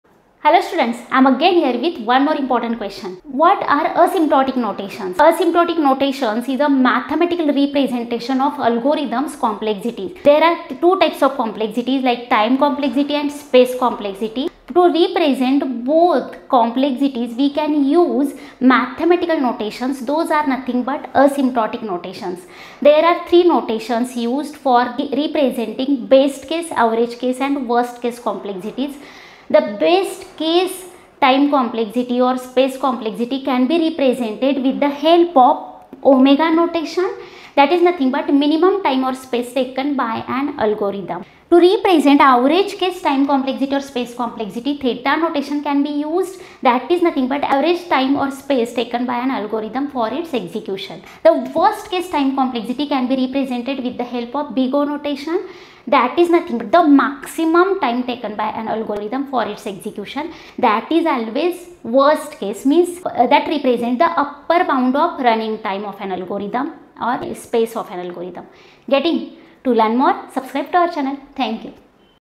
What are Asymptotic Notations? | Data Structures & Algorithms | eLogic Infotech
In this YouTube Shorts video by eLogic Infotech Classes, learn What are Asymptotic Notations in Data Structures and Algorithms (DSA) with easy and clear explanation.

Asymptotic Notations help us analyze the time and space complexity of algorithms. We cover the basics of:
 Big O Notation
 Omega Notation
 Theta Notation

These notations are crucial for optimizing your code and cracking technical interviews.

 Perfect for: Engineering students, BCA, MCA students & coding beginners.

 Follow us for more coding content & quick concept shorts